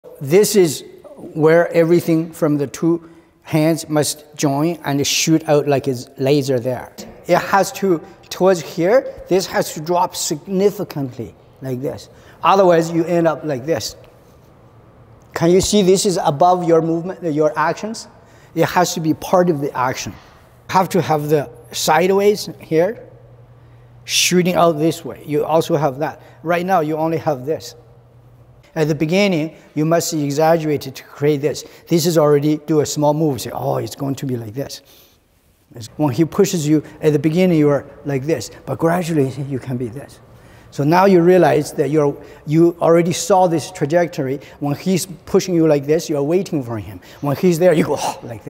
All To One Trailer-Chen Zhonghua Online 20211124
All body parts must focus towards the front hand. Master Chen showed many different ways and adjustments to make this possible.